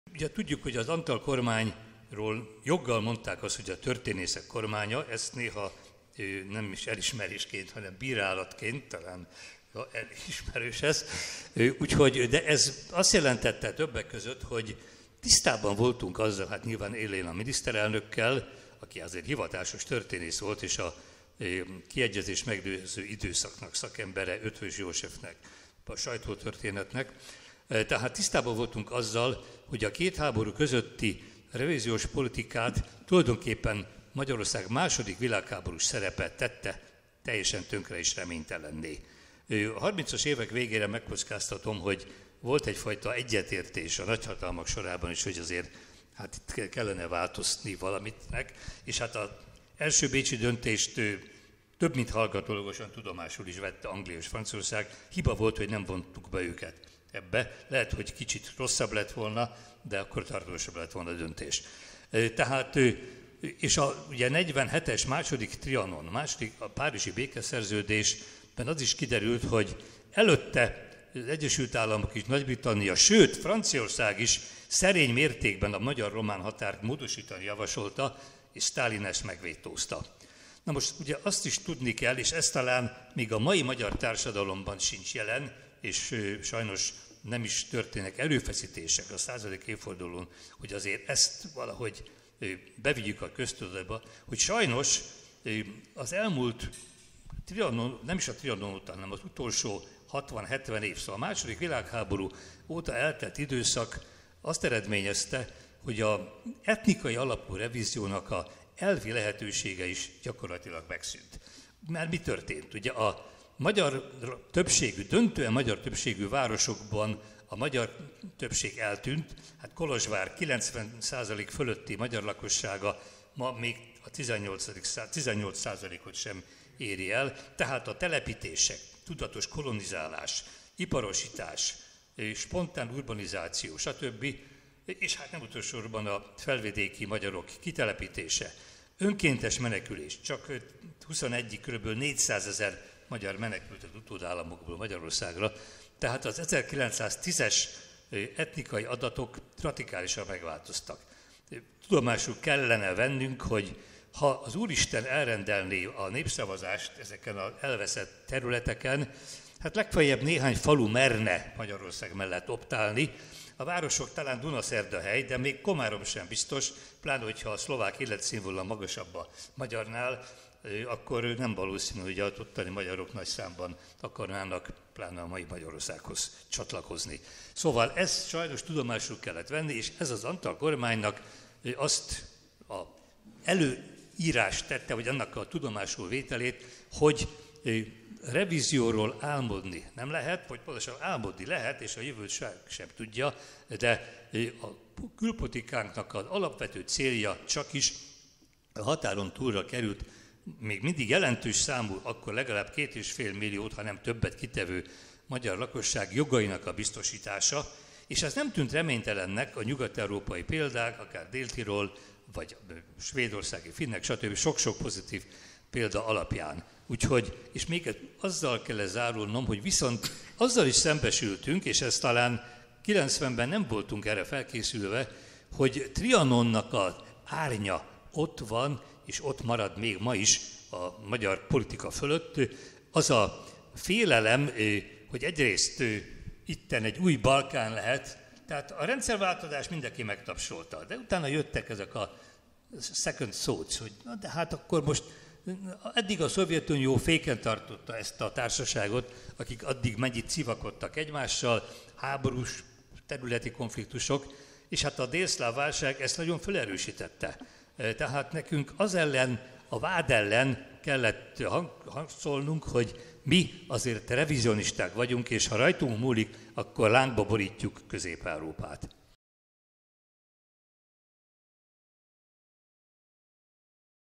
Az etnikai alapú revízióról Jeszenszky Géza álláspontja
"Az etnikai alapú revizíónak az elvi elehetőséges is gyakorlatilag megszűnt… Az 1910-es etnikai adatok radikálisan megváltoztak. … A külpolitikánknak az alapvető célja csak is a határon túlra került - még mindig jelentős száma, akkor legalább két és félmillió, ha nem többet kitevő - magyar lakosság jogainak a biztosítása."

Az etnikai alapú revízióról és Trianon árnyáról Dr. Jeszenszky Géza fejtette ki véleményét a Modell és Valóság előadássorozat X. évadának "Az Antall-kormány Trianon meghaladásáért" című rendezvényén 2020. október 22-én. Beszélgetőtársa Dr. Szent-Iványi István volt, Gyulai György moderálása mellett.